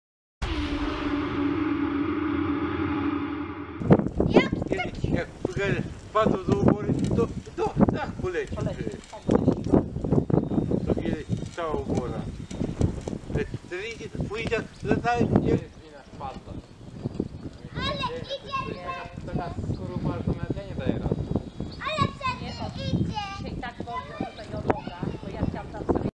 PIASKOWE TORNADO POD WŁODAWĄ
Witajcie ! Dziś trochę inny film niż zwykle, ale nie mogłem się powstrzymać ! Piękny diabełek pyłowy, zobaczcie sami ! Film nagrany w województwie Lubelskim.